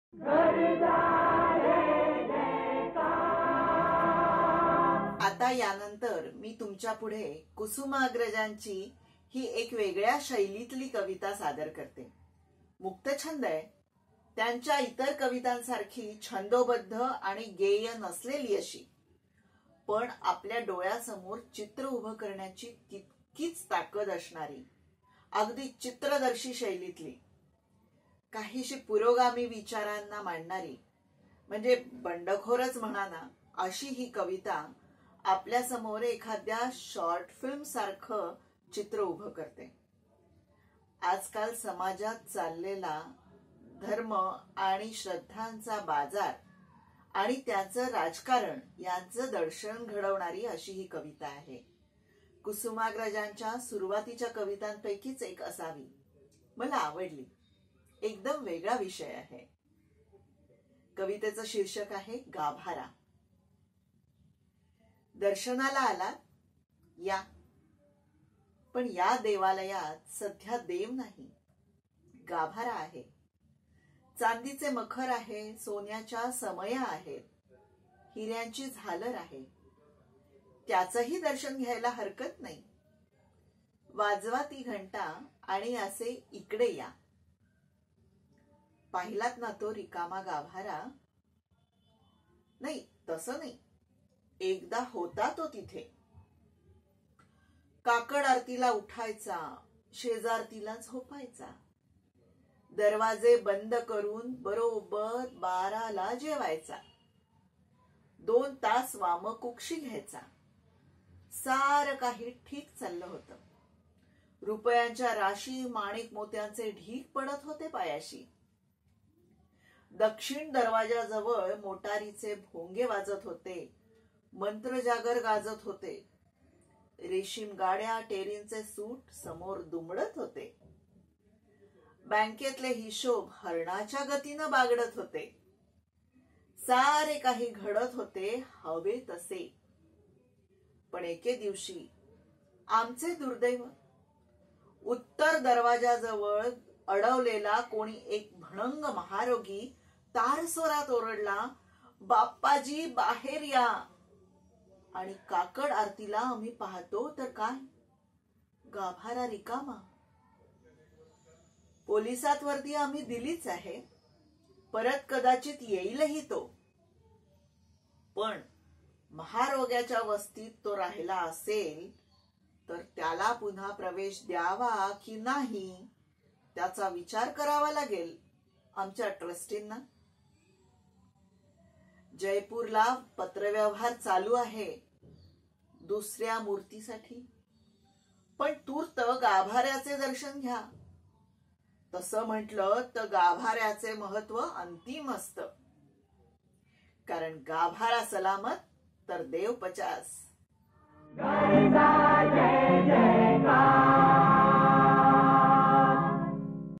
गाभारा | Gabhara - Kusumagraj Kavita by Ashwini Ghanvatkar मराठी DIVA आपले VI.VA. | मराठी भाषा दिवस
गाभारा Gabhara - Kusumagraj by Samiksha Salunke
मराठी DIVA आपले VI.VA. | मराठी भाषा गौरव दिन | कुसुमाग्रज जन्मदिन | कुसुमाग्रजांच्या कवितांचे वाचन

कलाकार : आश्विनी घनवटकर | Artist : Ashwini Ghanvatkar
संकलन : समिक्षा साळुंके | Edit : Samiksha Salunke